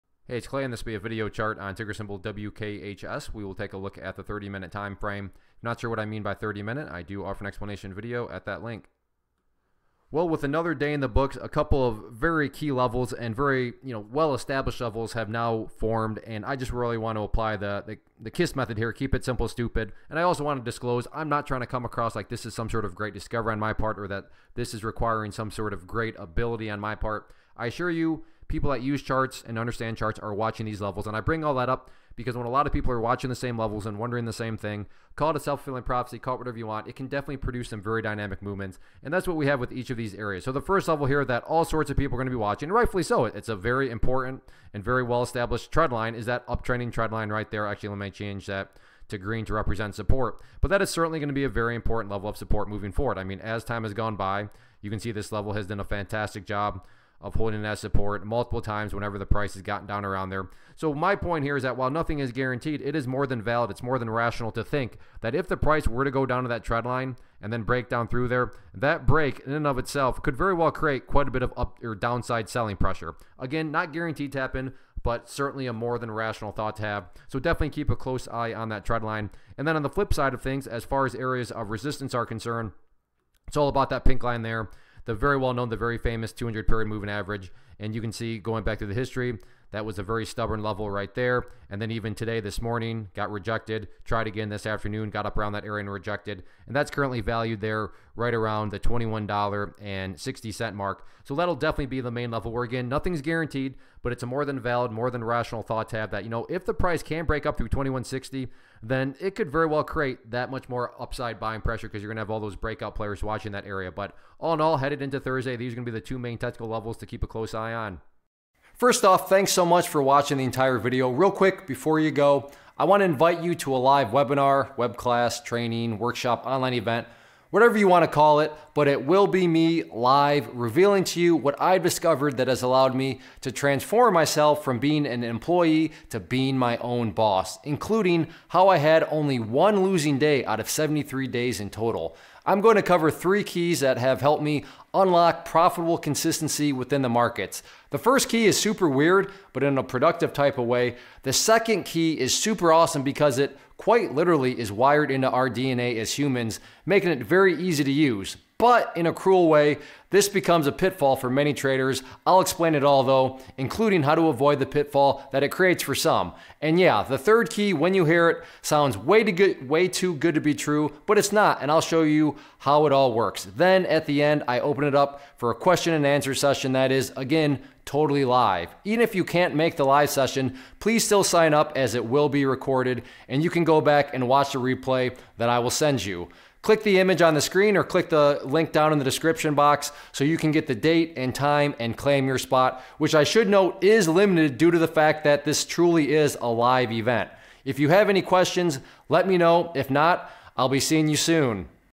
Workhorse Group Inc. - WKHS Stock Chart Technical Analysis for 12-30-2020
Learn how to read stock charts and identify technical patterns as ClayTrader does a quick stock chart review on Workhorse Group Inc. (WKHS).